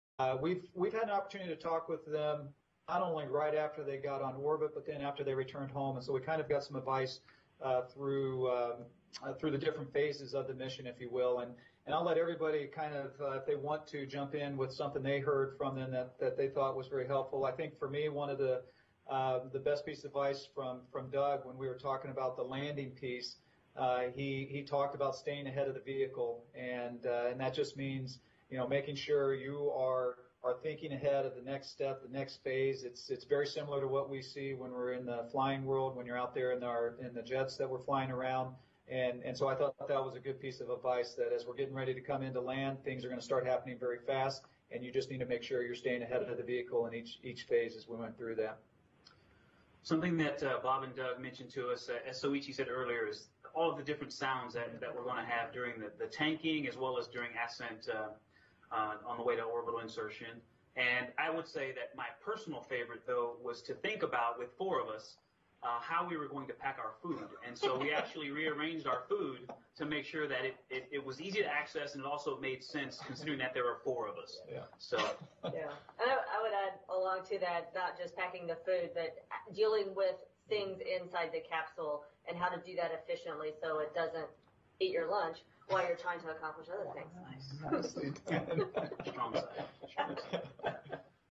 What advice did SpaceX Demo-2 crew give to Crew-1 astronauts?
NASA's Bob Behnken and Doug Hurley talked to the SpaceX Crew-1 astronauts about their flight. Hear some of the advice they gave to the crew that will launch the first operational mission of the SpaceX Crew Dragon era.

Credit: NASA